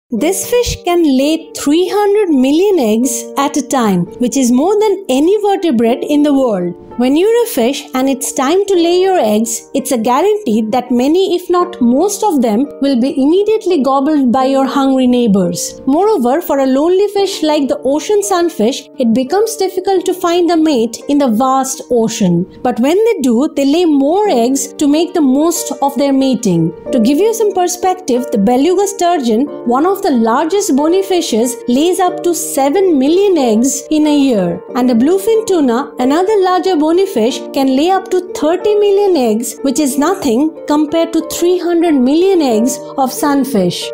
This Fish Can Lay 300 Million Eggs At A Time!
Did you know there is a fish that can lay 300 million eggs at a time, which is more than any vertebrate in the world! Let's find out!